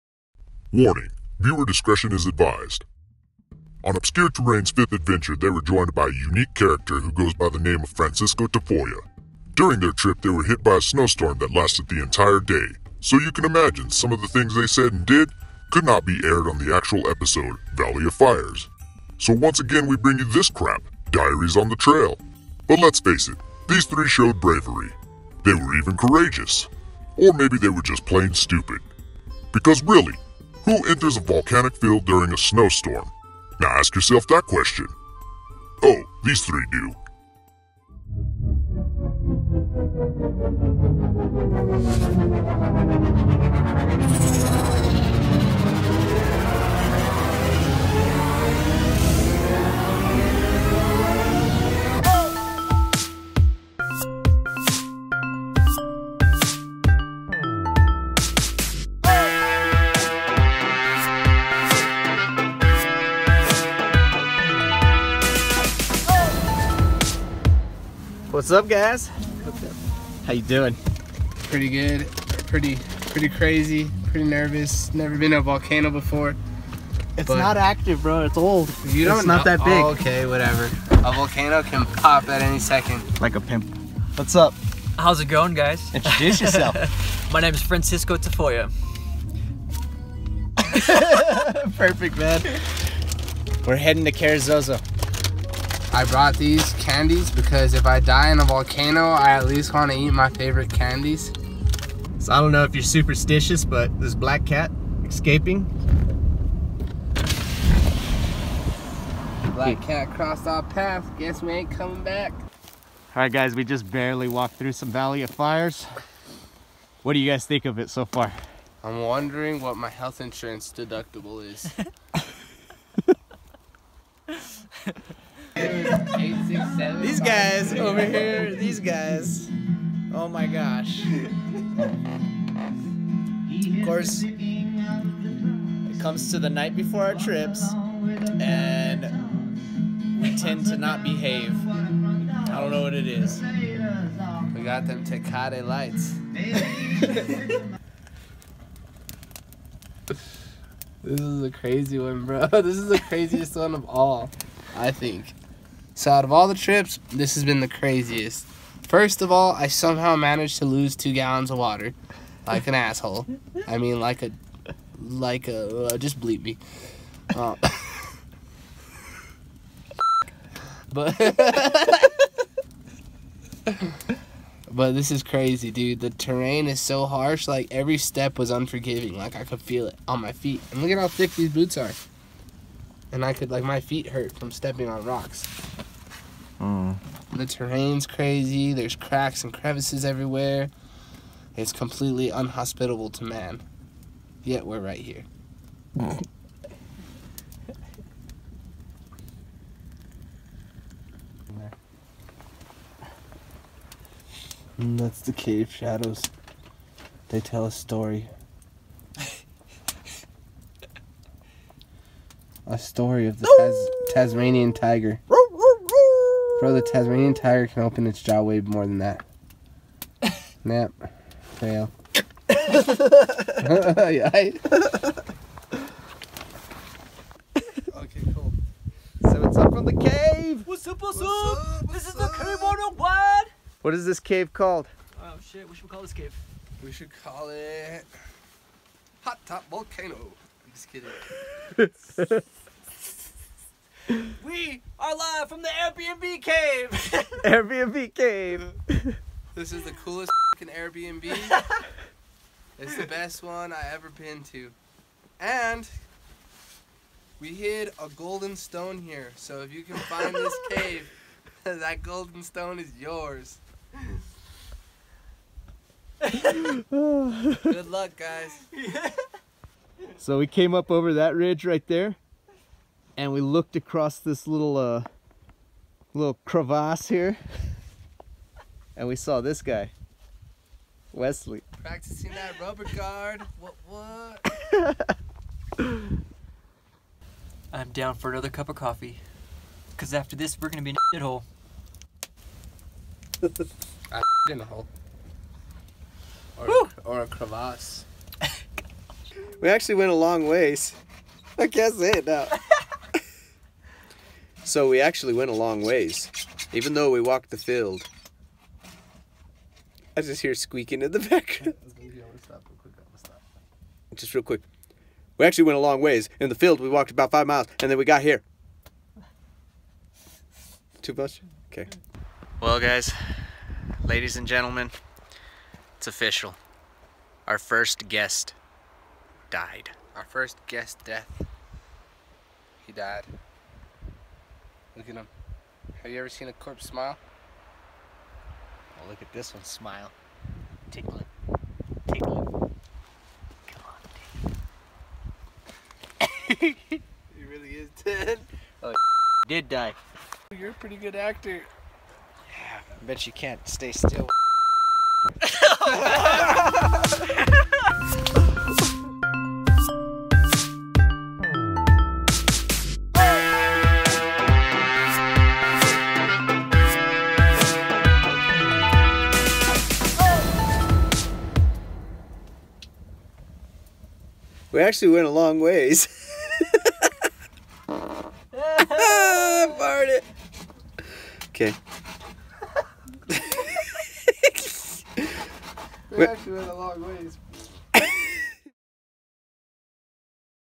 We All Say & Do Dumb things!!!! Diaries on the Trail
Hello Everyone,
So once again we are releasing all the stupid stuff that happens on the trail. This is the stuff we don't put on the actual episode because we are some goofy guys. We hope you enjoy this random footage.
Also go follow and like Francisco's project "Drifter"

Go follow Drifter on Instagram & FaceBook!